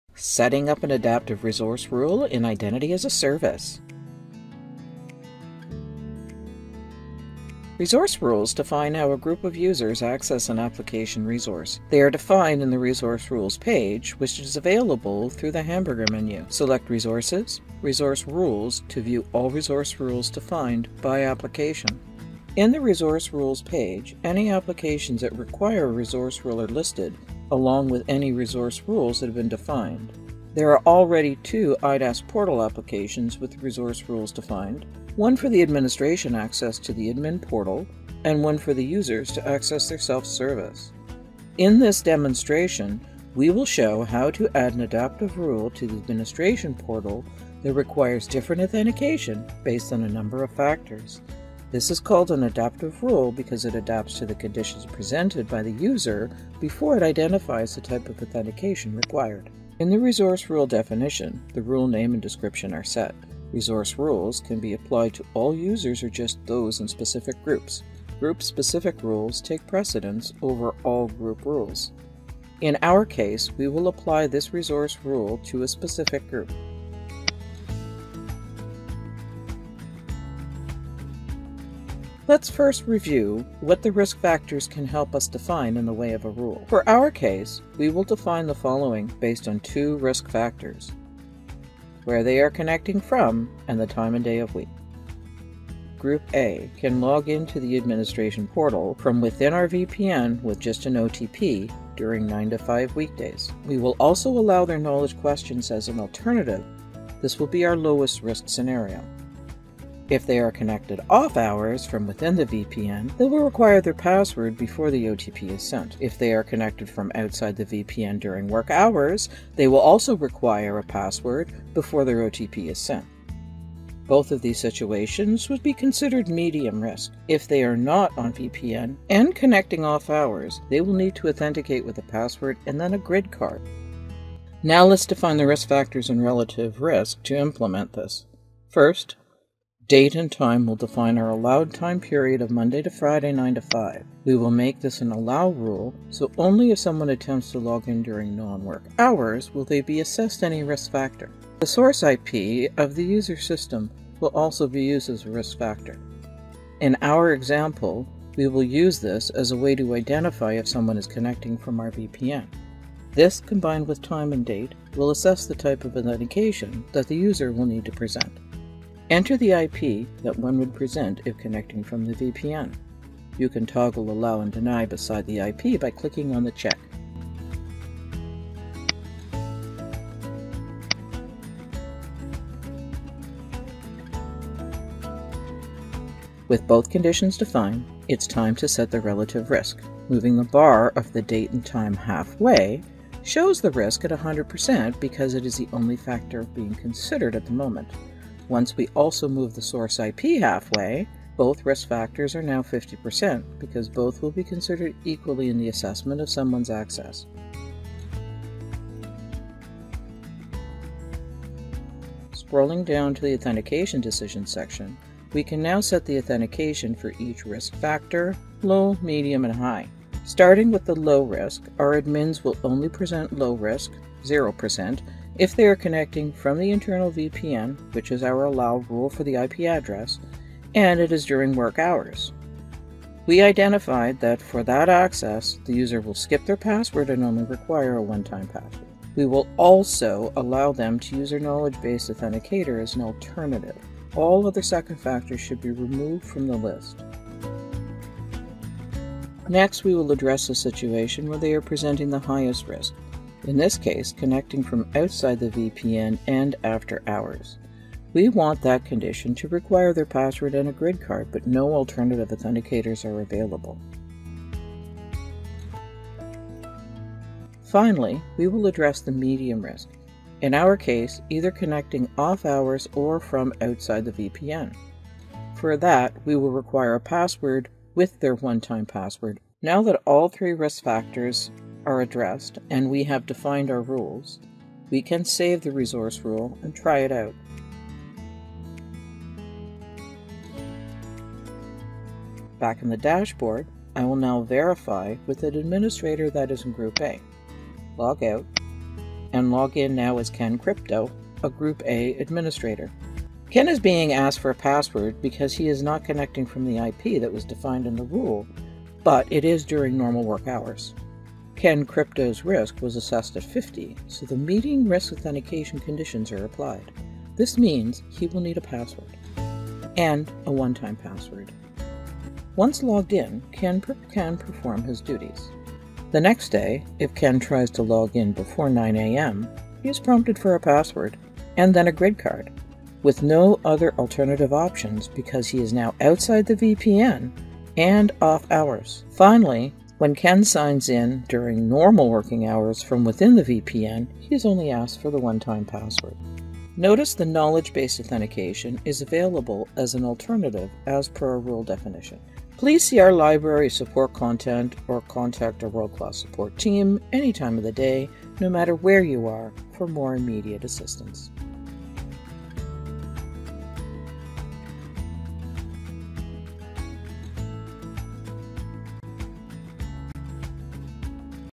Entrust Identity as a Service: Setting Up an Adaptive Resource Rule
Learn how to set up an adaptive resource rule.

From desktop login and single sign-on to privileged access, the Entrust Identity as a Service cloud authentication solution enables existing and future use cases for all users — including employees, partners, contractors and customers.